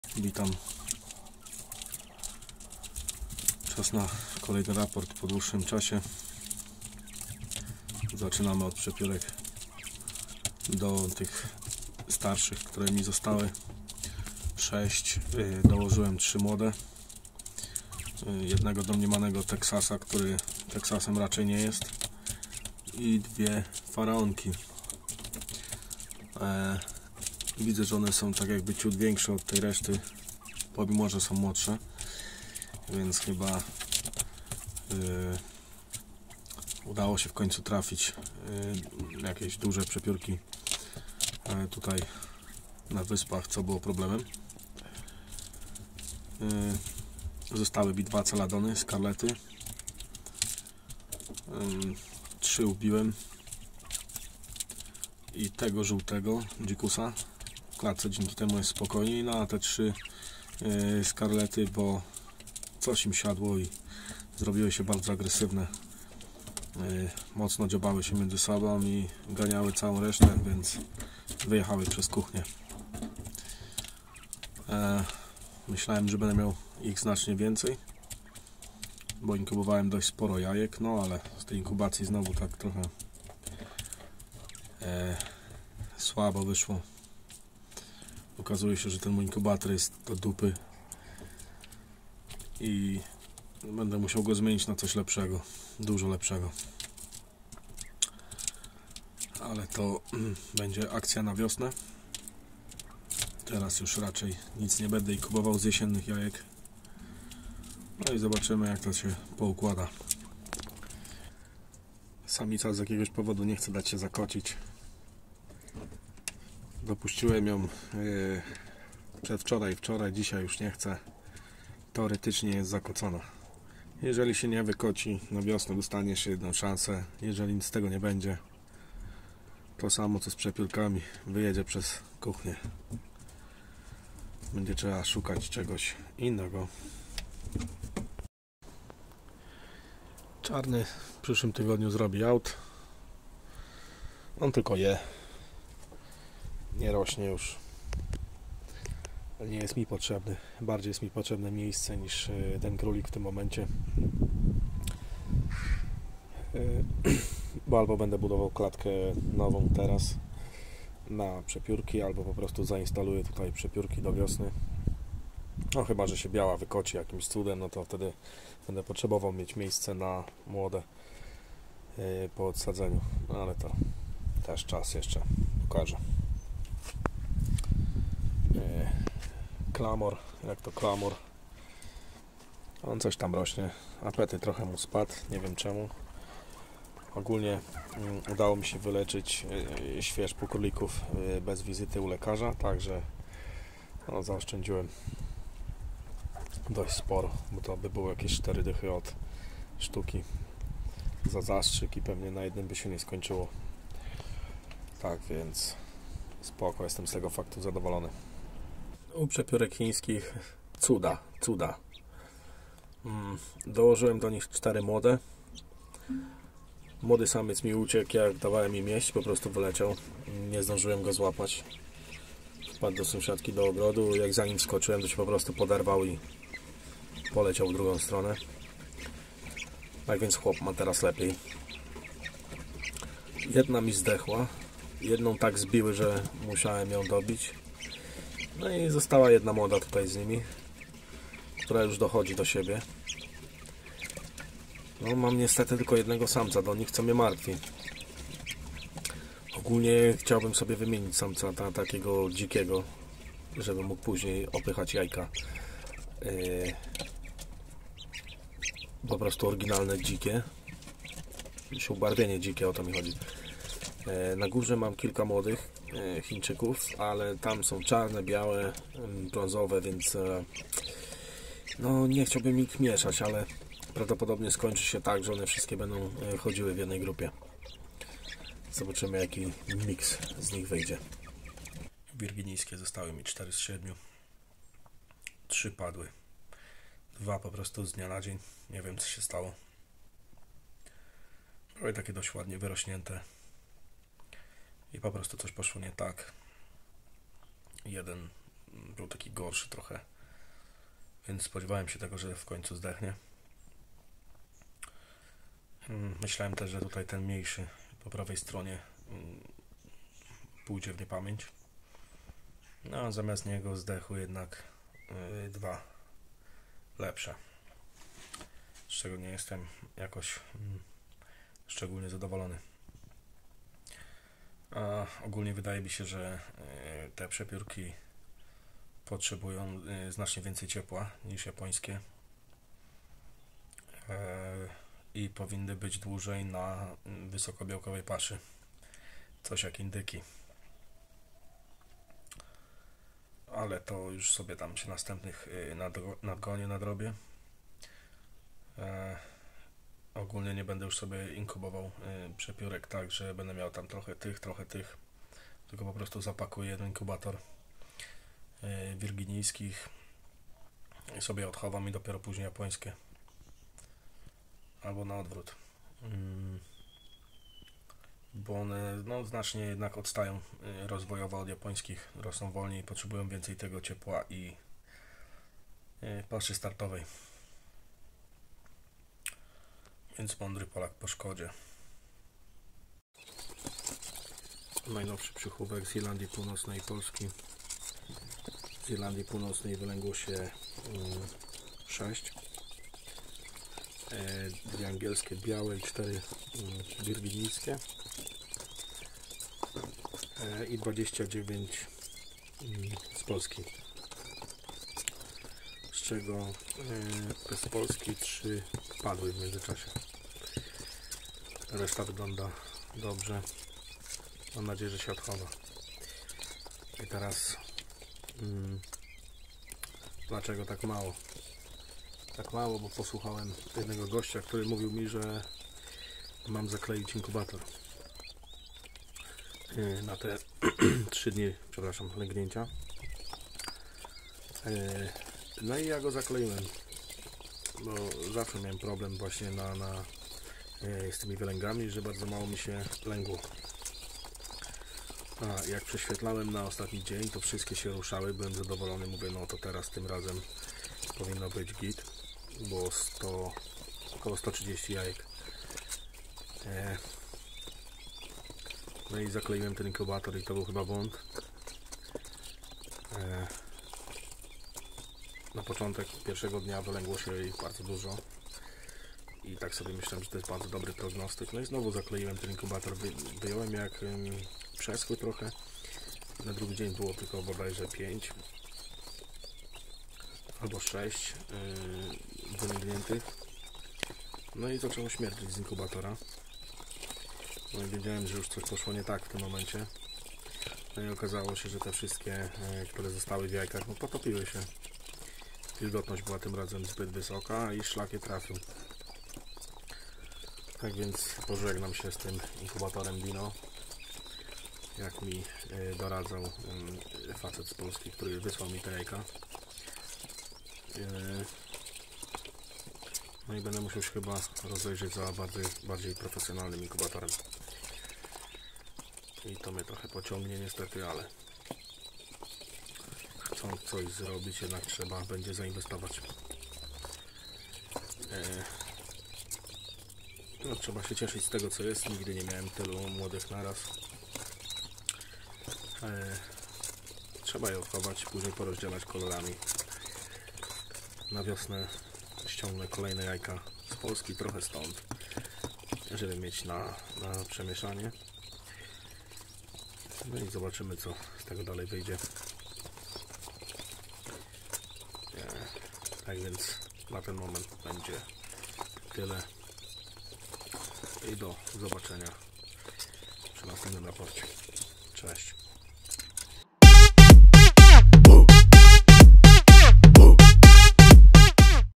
Kolejne lęgi przepiórek i raport
Witam. Dzisiaj publikuję raport z mojej małej hodowli. Samica królika znowu się nie wykociła i z niej raczej już nic nie będzie. Przetrzymam ją do wiosny i zobaczę czy dziewczyna przemysli sprawę bo zaczyna się kwalifikować do pieczeni. Kolejme przepiórki się wylęgły co pozostawię bez większego komentarza. Zapraszam do oglądania i pozdrawiam